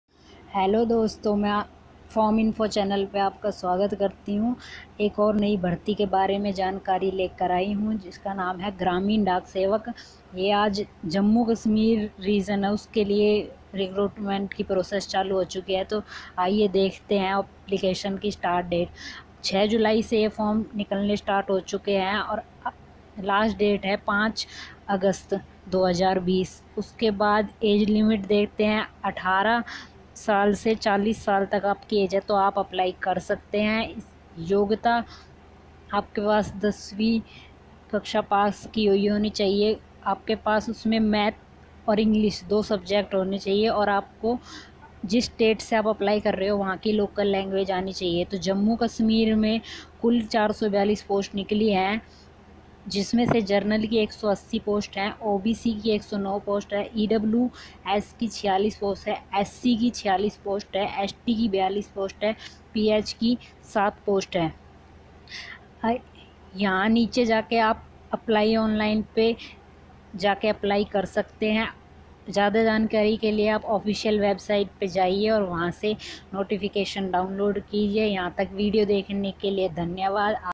Gramin dak sevak recruitment in Jammu and kashmir
Gramin dak sevak ki bhartiya dak vibhag m total 442 post nikli h .